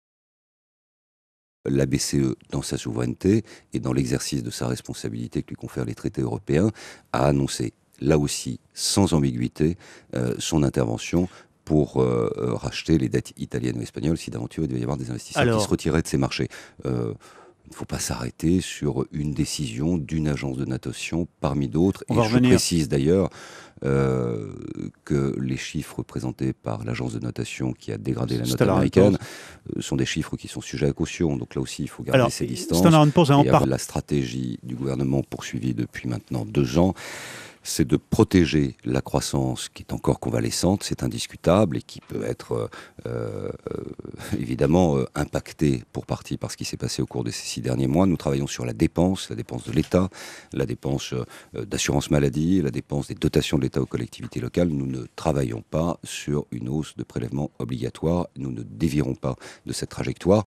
La BCE prête à racheter la dette espagnole et italienne
La Banque centrale européenne (BCE) est prête à racheter de la dette espagnole et italienne, si des investisseurs se retirent, a indiqué lundi 8 août le ministre français de l'Economie et des Finances François Baroin sur la radio privée Europe 1. Durée: 01:00